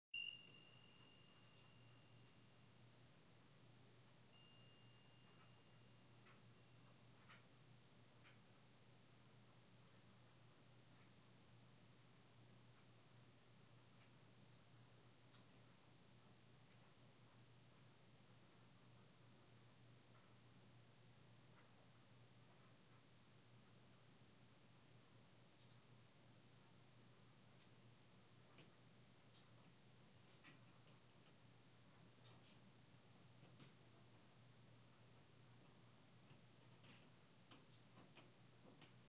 CHIMES AND LIGHT ANOMALIE 😱
We heard the chimes earlier do checked the security footage and not only caught chimes again but watch the stange yet clear light anomalie (Orb) to the left by the tree